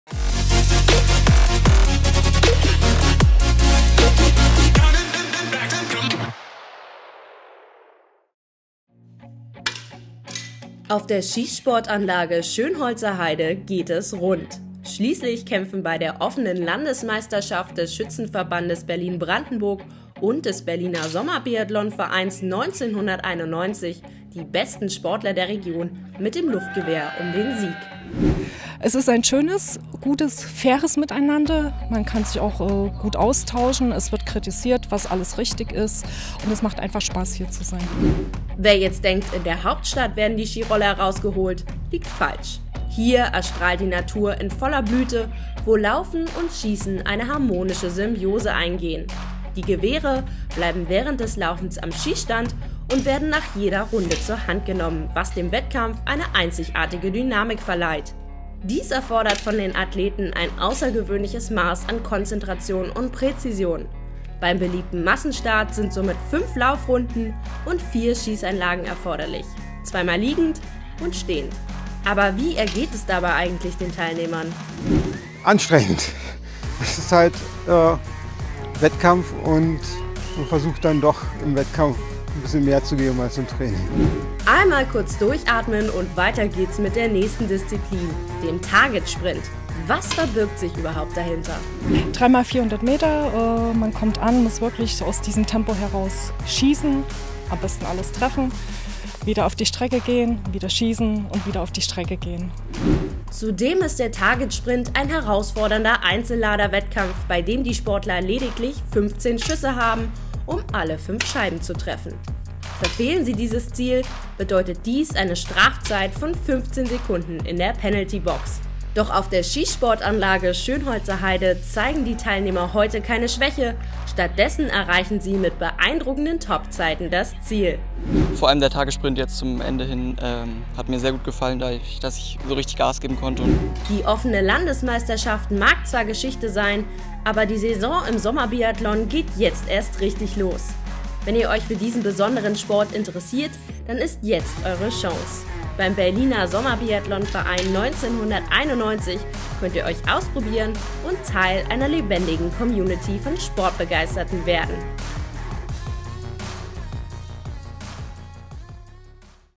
ZIELGERICHTET ZUM ERFOLG: Offene Landesmeisterschaft - Sommerbiathlon Luftgewehr
Auf der Schießsportanlage Schönholzer Heide geht es rund! Schließlich kämpfen bei der Offenen Landesmeisterschaft des Schützenverbandes Berlin-Brandenburg e.V. und des Berliner Sommerbiathlon Vereins 1991 e.V. die besten Sportler der Region mit dem Luftgewehr um den Sieg.